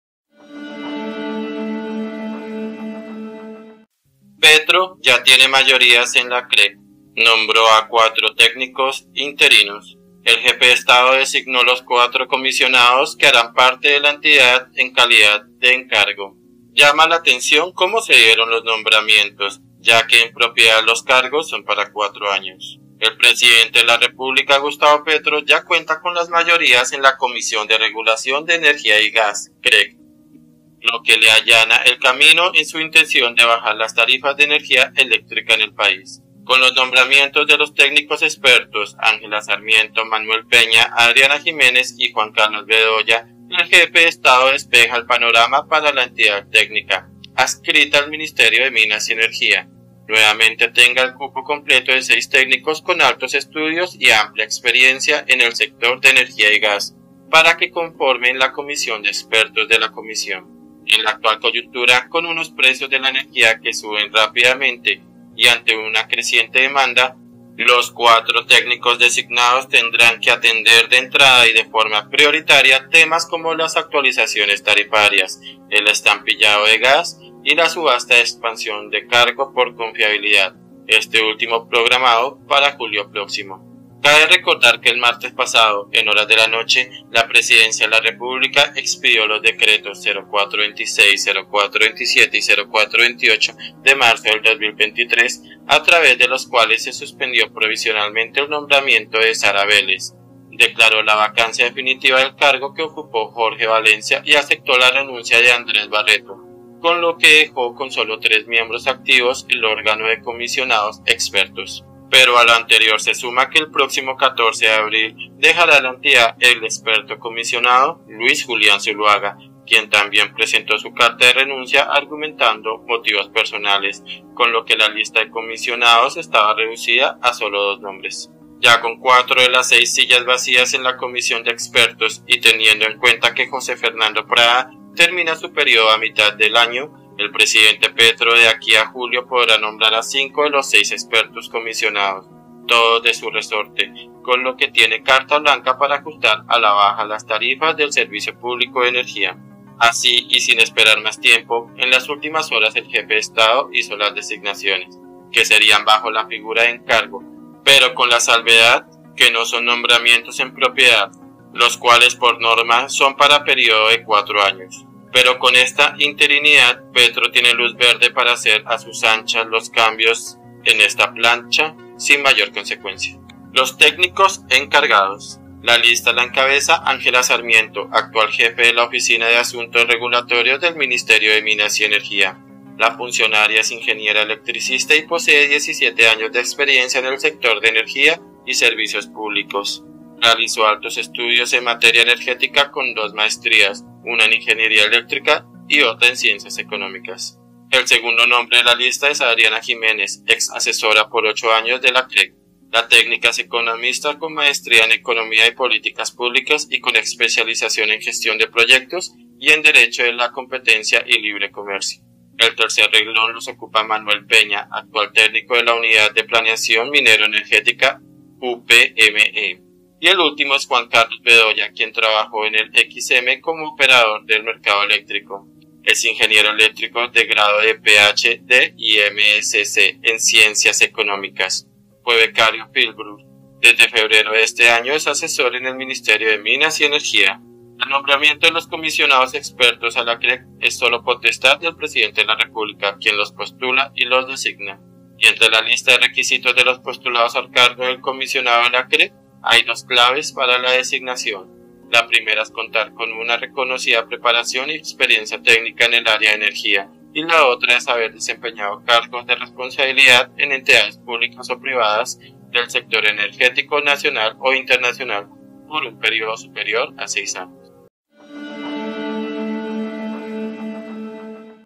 ALERTA ⚠️ PETRO YA TIENE MAYORIAS EN LA CREG
Petro ya tiene mayorías en la Creg: nombró a cuatro técnicos interinos
El jefe de Estado designó los cuatro comisionados que harán parte de la entidad en calidad de encargo. Llama la atención cómo se dieron los nombramientos, ya que en propiedad los cargos son para 4 años.